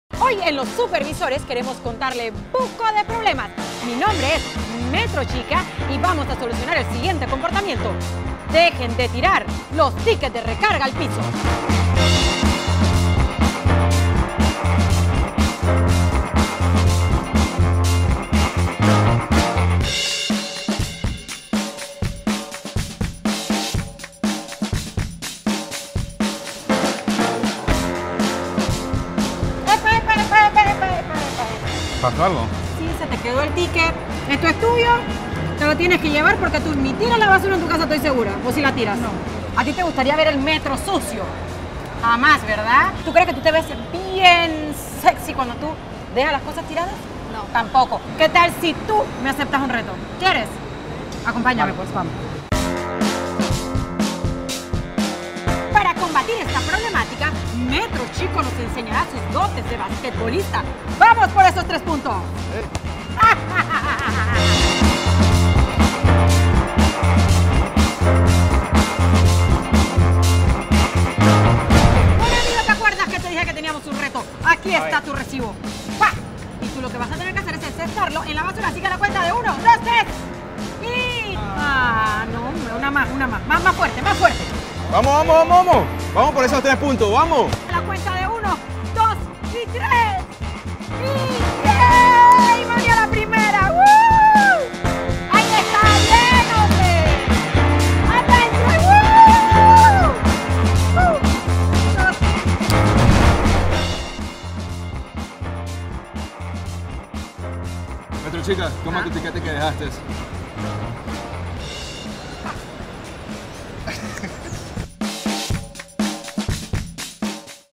Los Supervisores | Llévate tu comprobante de recarga, no lo dejes
Cuando uses las máquinas para compra o recarga de tarjetas que se encuentran en las estaciones del Metro de Panamá, y pides tu comprobante no lo dejes. si lo dejas, se cae al suelo y se genera basura.
Contribuye a Mantener nuestro Metro limpio.